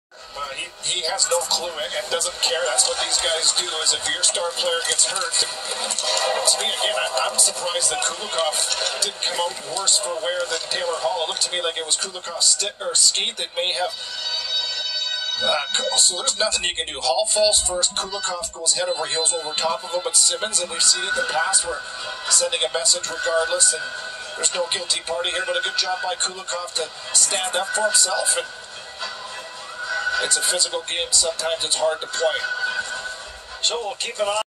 Taylor Hall collides with Kulikov
Taylor Hall Kulikov Winnipeg Jets new jersey Devil's November 5 2019